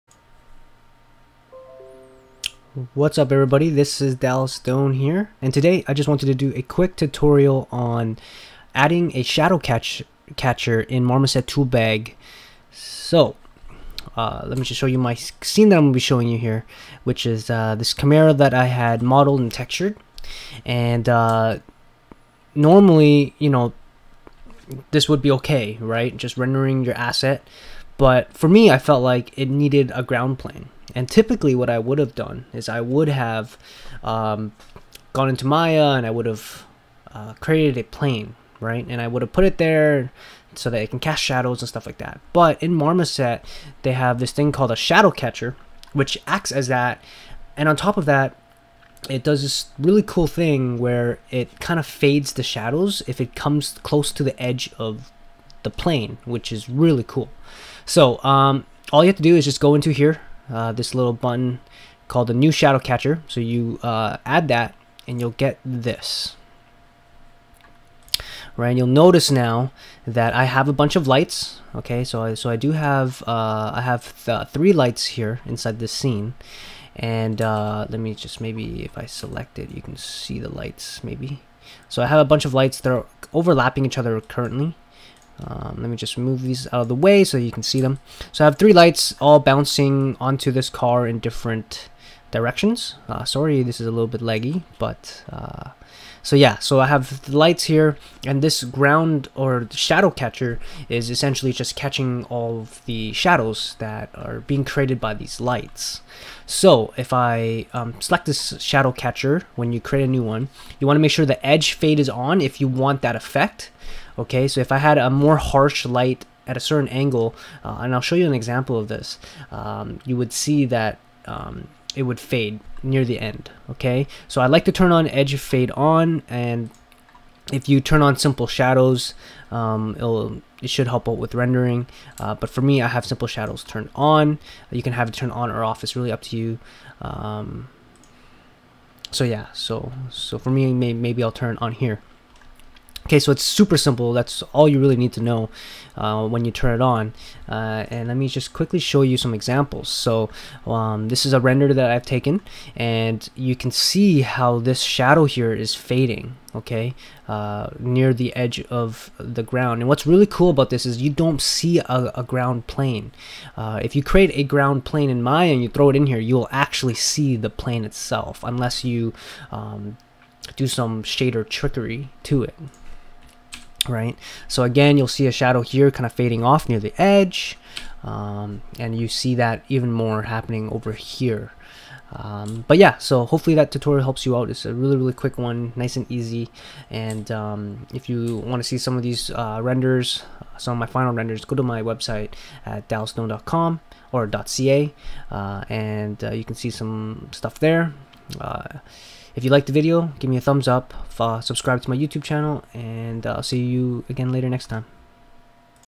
Marmoset Toolbag 3 Tutorial: Adding Shadow Catcher or Ground Plane Quick Tutorial
Have you ever wondered how to add a nice ground plane or shadow catcher in your Marmoset Toolbag Scene? Here is a quick tutorial on how to do just that.

I want to help other artists achieve their dreams of working in video games by creating Tutorials and Videos.